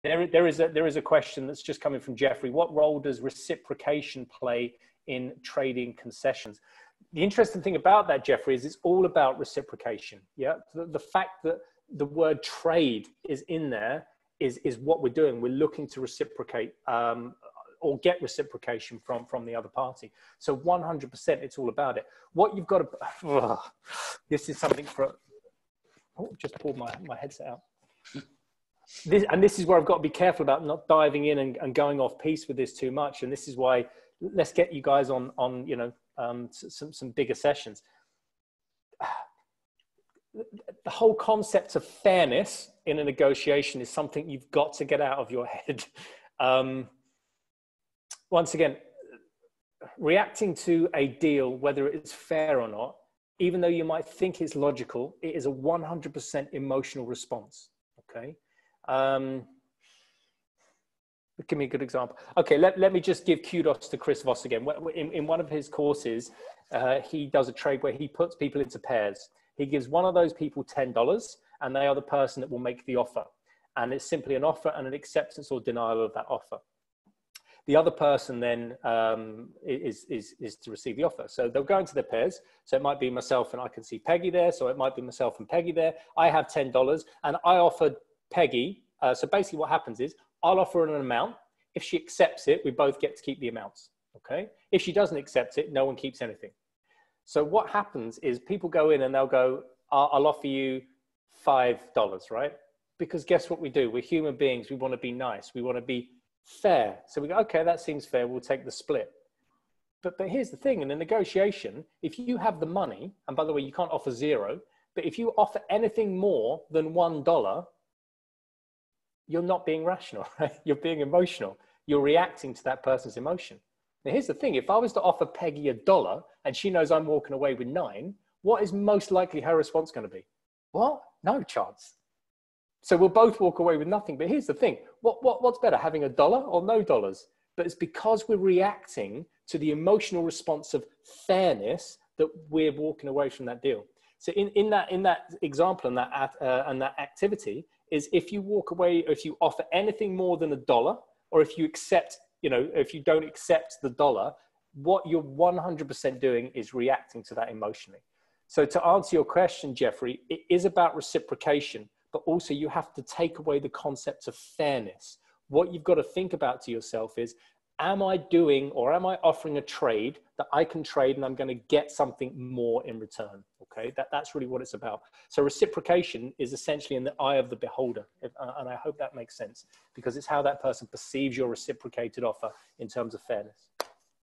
Q&A: Discussing reciprocation and fairness in negotiating
A 3-min segment where our lead consultant Russell Overy answers the question  'What role does reciprocation play in trading concessions?' in our webinar series from 2020.